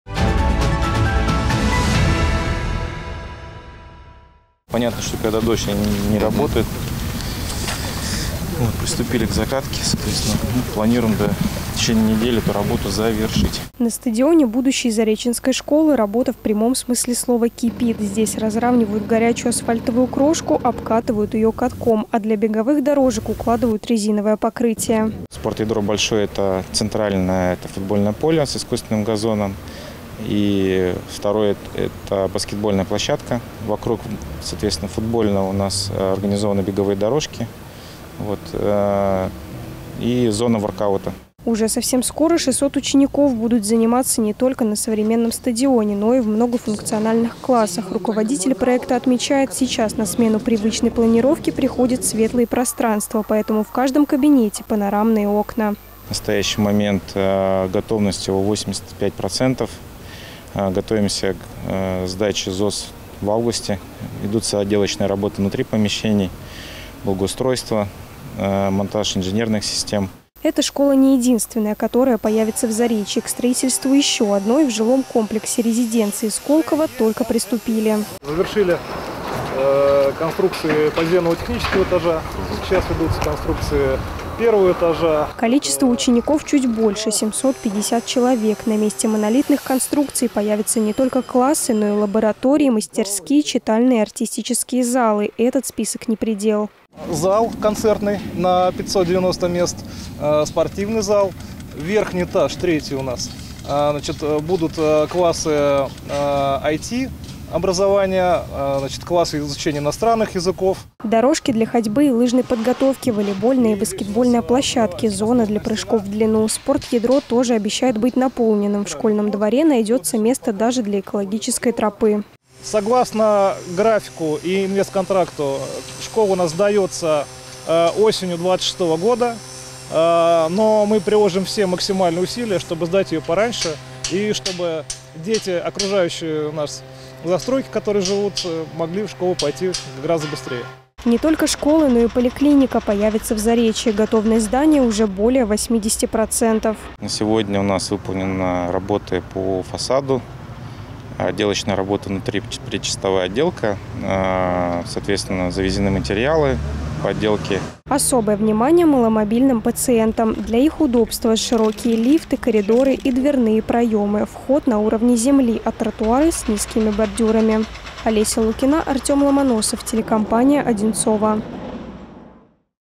Перемены в Заречье
В Заречье одновременно возводят несколько социально важных объектов - две школы и поликлинику. О готовности зданий и сроках их сдачи расскажет Олеся Лукина.